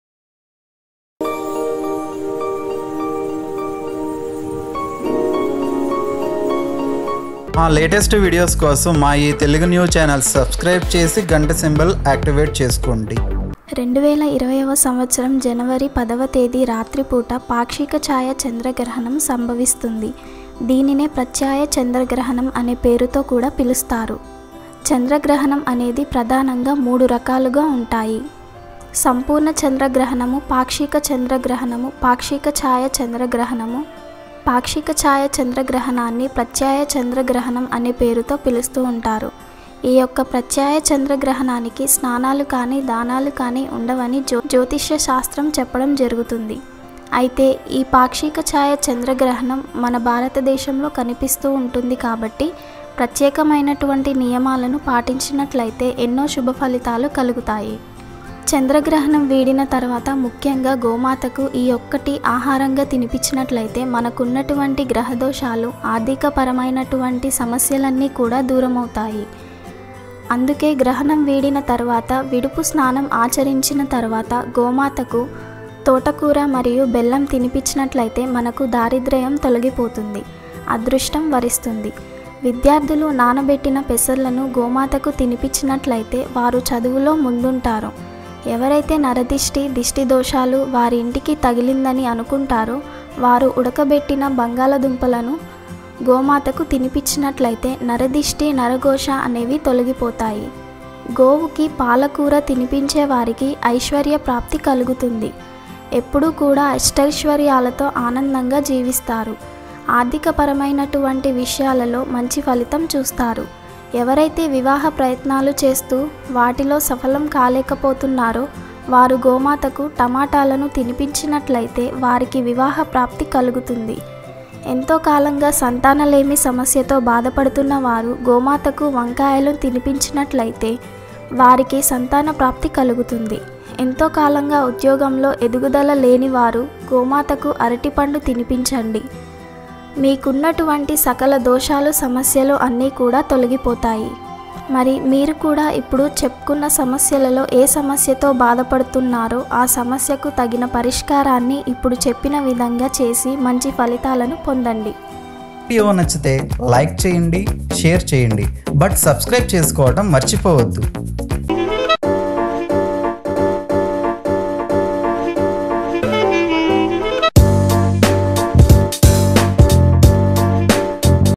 రేపు శనివారం గ్రహణం మరుసటి రోజు ఇది గోమాతకు తినిపిస్తే మీ జీవితం మారి కోట్లకు పడగలెత్తుతారు
Telugu New